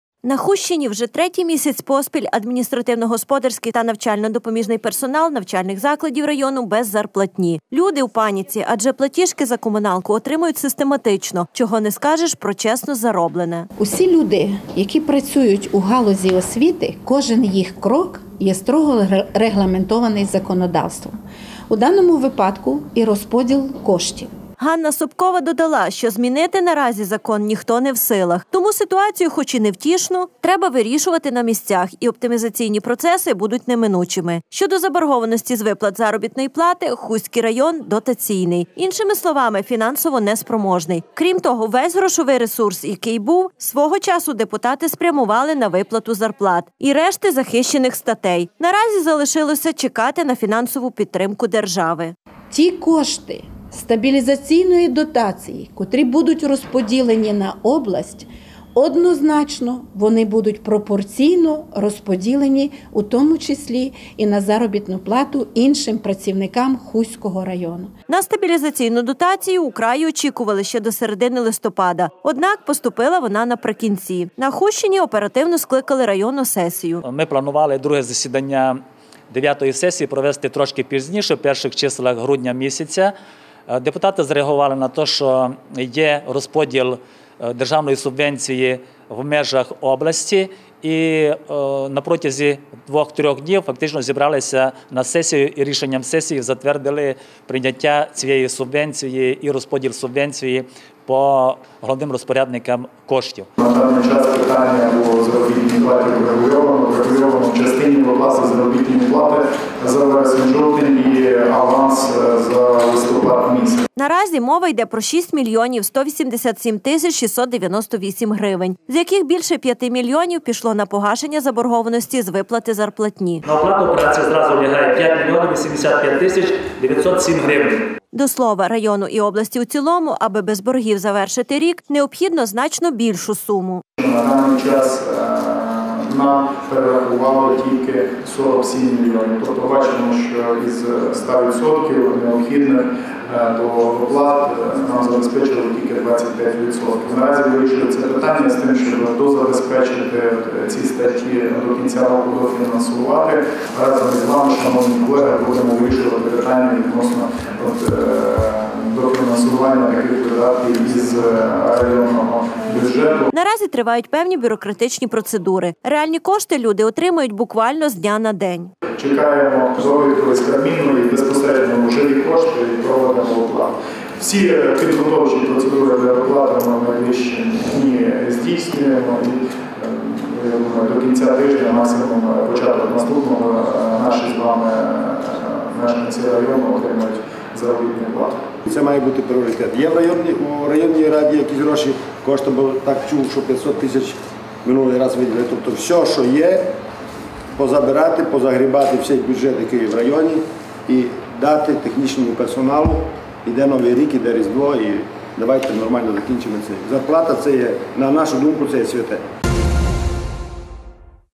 Погашення заборгованості
На Хустщині вже третій місяць поспіль адміністративно – господарський та навчально – допоміжний персонал навчальних закладів району без зарплатні. Люди у паніці, адже платіжки за комуналку отримують систематично, чого не скажеш про чесно зароблене. Ситуацію, хоч і невтішну, треба вирішувати на місцях і оптимізаційні процеси будуть неминучими. Щодо заборгованості з виплат заробітної плати, Хустський район - дотаційний. Іншими словами фінансово неспроможний. Крім того, увесь грошовий ресурс, який був - свого часу депутати спрямували на виплату зарплат і решти захищених статей. Наразі залишалося - чекати на фінансову підтримку держави. На стабілізаційну дотацію у краї очікували ще до середини листопада, однак поступила вона наприкінці. На Хустщині оперативно скликали районну сесію. Наразі мова йде про 6 млн. 187 тисяч 698 грн., з яких більше 5-ти мільйонів пішло на погашення заборгованості з виплати зарплатні. До слова, району і області у цілому, аби без боргів завершити рік, необхідно значно більшу суму. Наразі тривають певні бюрократичні процедури. Реальні кошти люди отримають буквально з дня на день. Чи зможуть у районі до кінця бюджетного року вишукати кошти для оплати праці технічних працівників ще за два місяці? – поки невідомо. Однак пообіцяли прикласти максимум зусиль, аби увійти у Новий рік, не боргуючи.